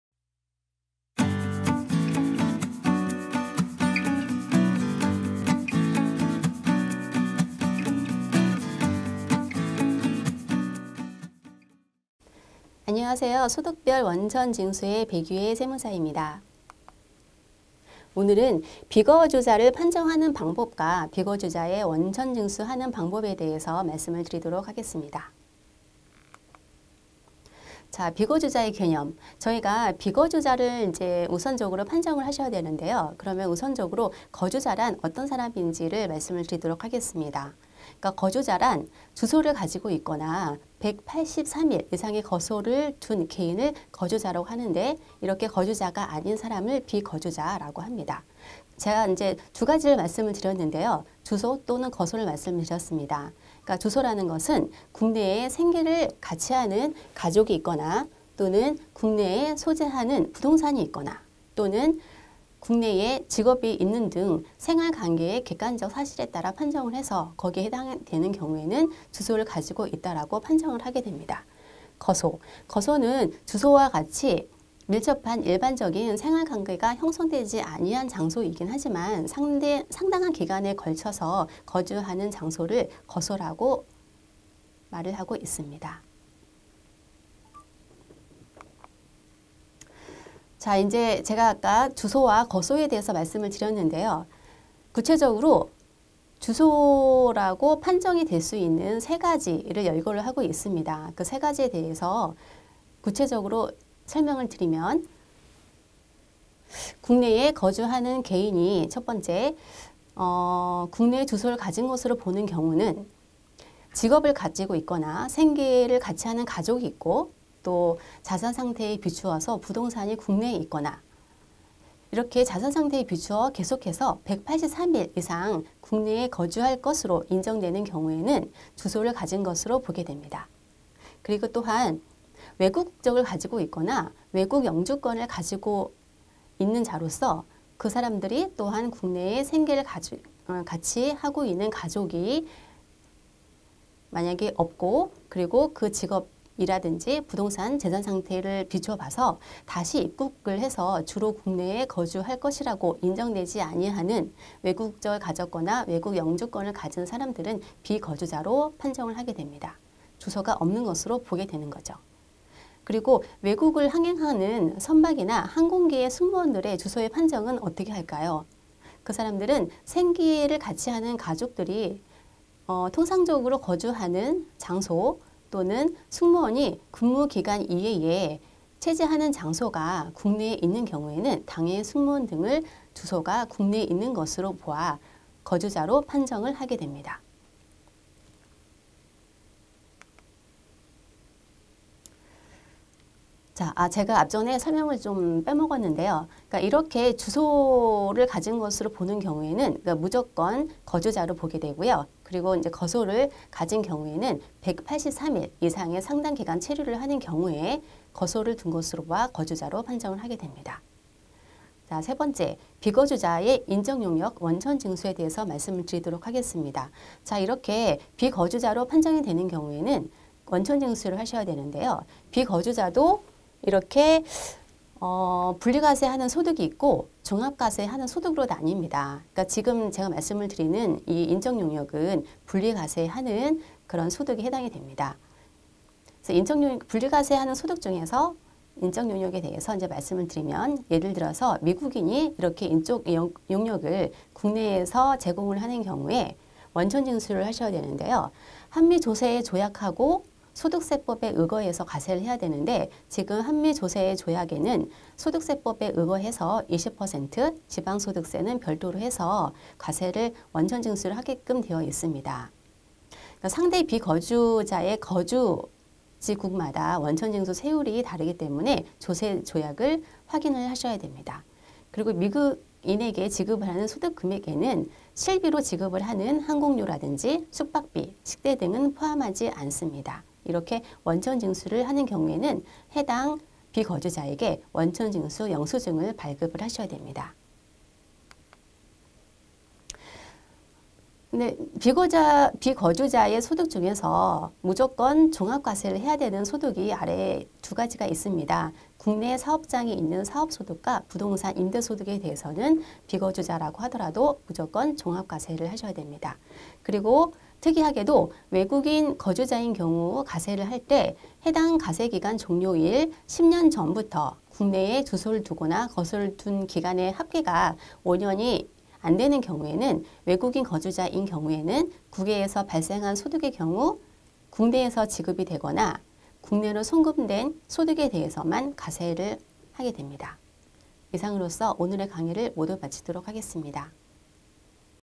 #11 비거주자의 국내 원천소득에 대한 원천징수 - 백유애 세무사의 5분특강
비거주자의 국내 원천소득에 대한 원천징수
1.비거자의개념
2.비거주자 인적용역 원천징수